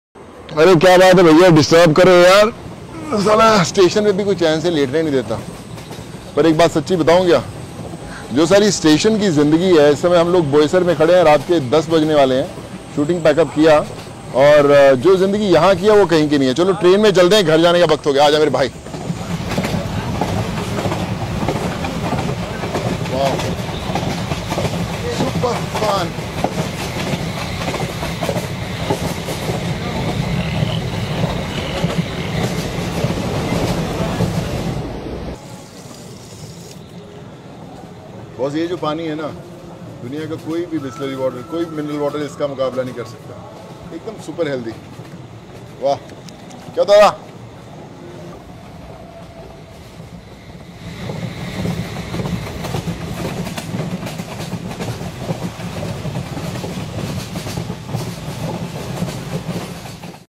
SonuSood Sonu Bollywood IndianRailway LocalTrain SocialMedia Viral ViralVideo
सोनू सूद ने सोशल मीडिया पर अपनी ट्रेन यात्रा का एक वीडियो शेयर किया है. सोनू कहते हैं कि यहां की जिंदगी जैसी होती है वैसी कहीं की नहीं है. वीडियो में सोनू रेलवे स्टेशन पर सोते हुए और नल से पानी पीते हुए भी नज़र आते हैं.